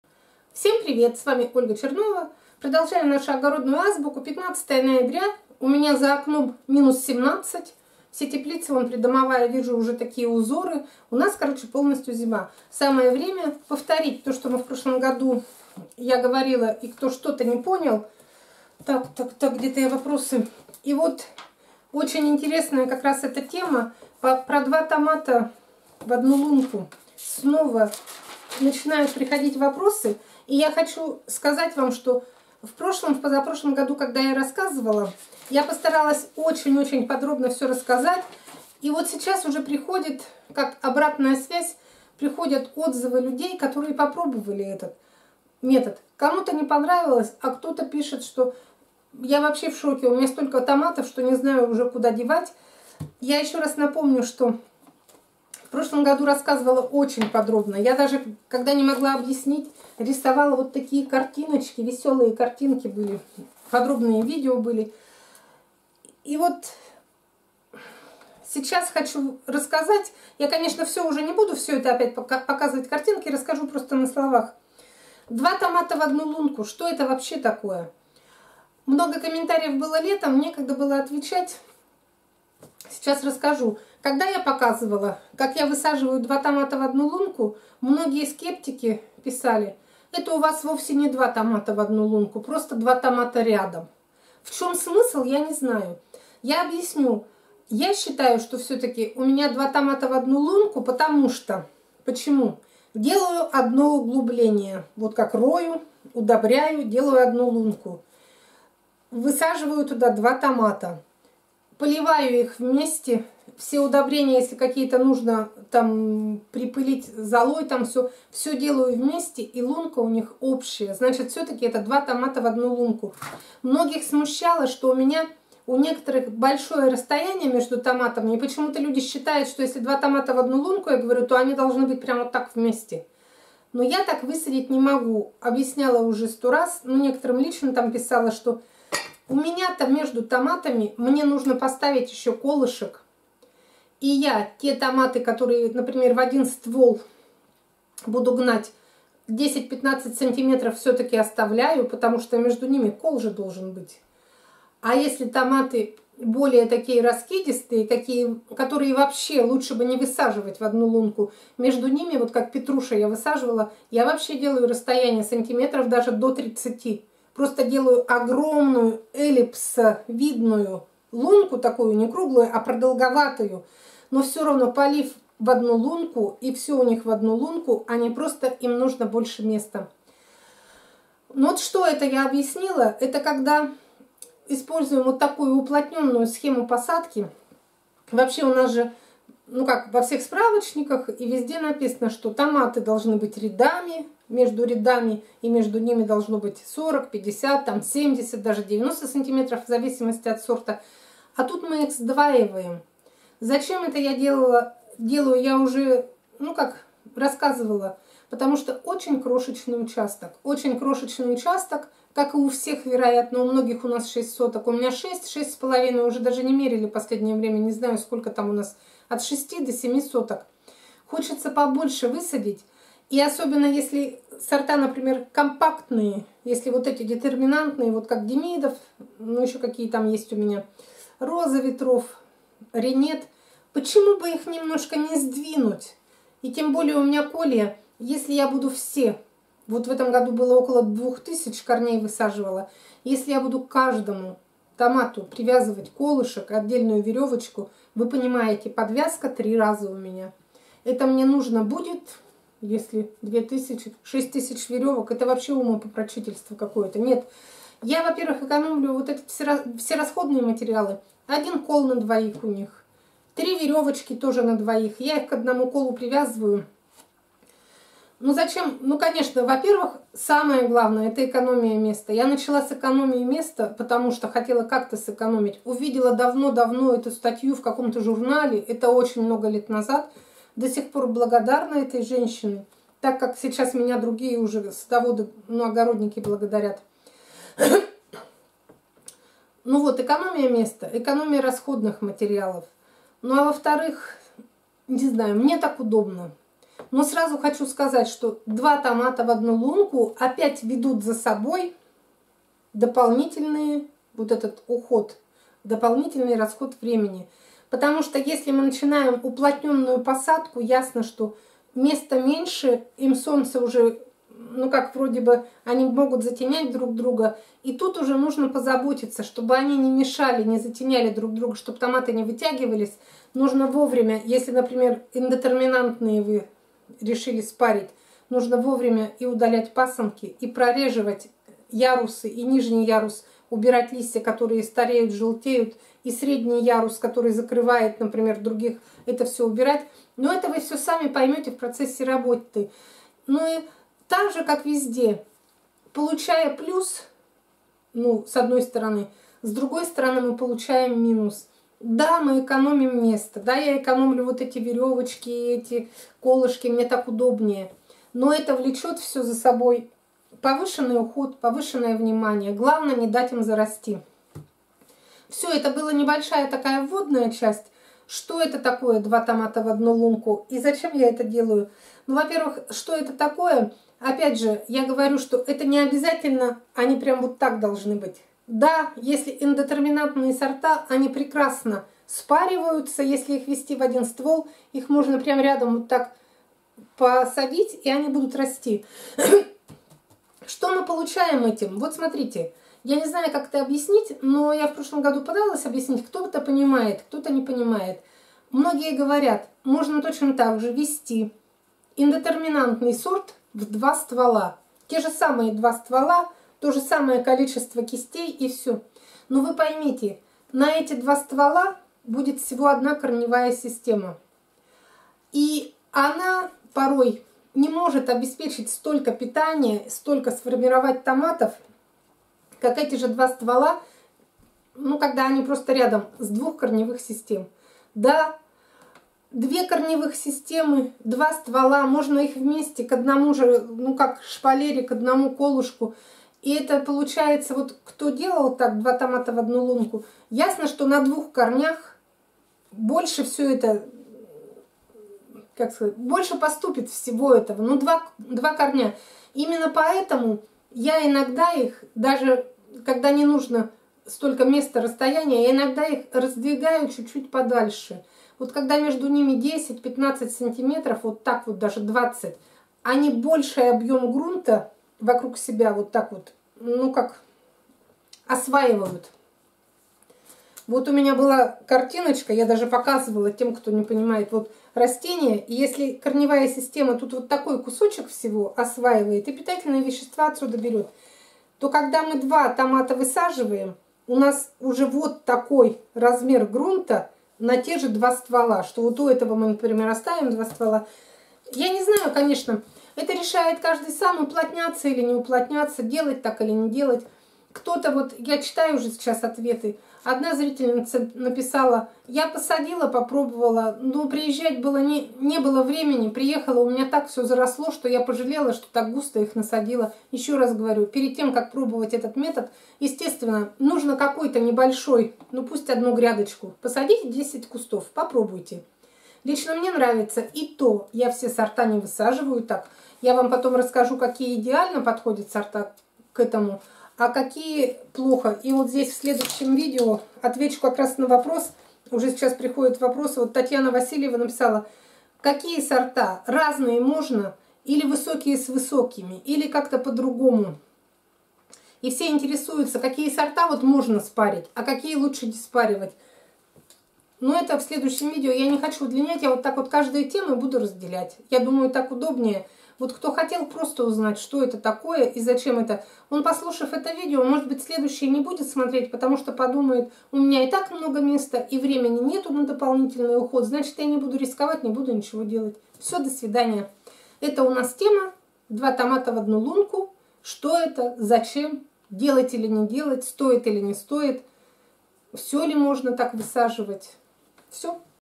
2 ТОМАТА В ОДНУ ЛУНКУ - ЧТО ЭТО И ЗАЧЕМ? Ольга Чернова.
Два томата в 1 лунку - это выражение слышали все. Но начинающие огородники не все знают что это такое. И зачем высаживать в 1 лунку по 2 томата. В этом видео об уплотненной схеме посадок  по два в одну лунку. На фото - томат Демидов. выращиваемый в теплице по два в одну лунку.